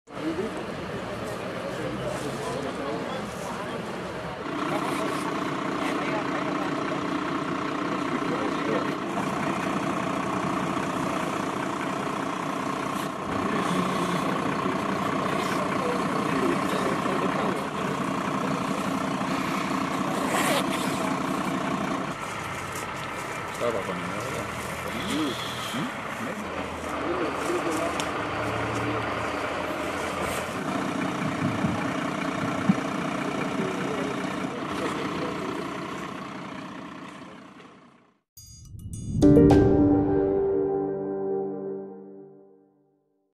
Drie gewonden naar ziekenhuis na frontale botsing in Enschede
Drie personen zijn vanavond gewond geraakt bij een frontale aanrijding op de Geesinkweg in Enschede. Twee auto's botsten frontaal op elkaar, waarbij er n auto in brand vloog.